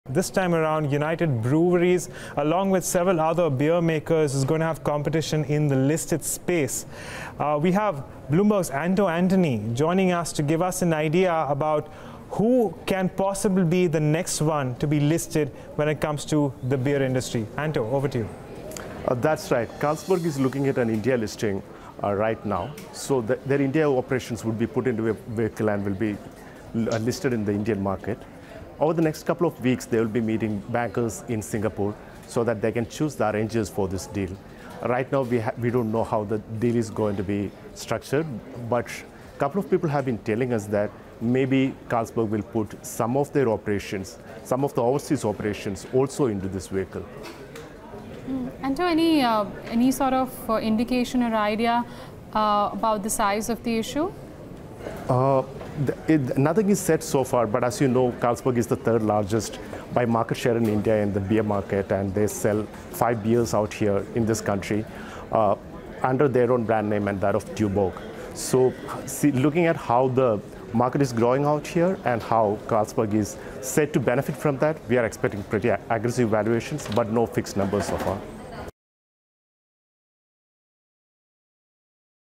Carlsberg India Headed For The Bourses
Carlsberg is planning a domestic IPO of its Indian business. Bloomberg's Anto Antony shares more details.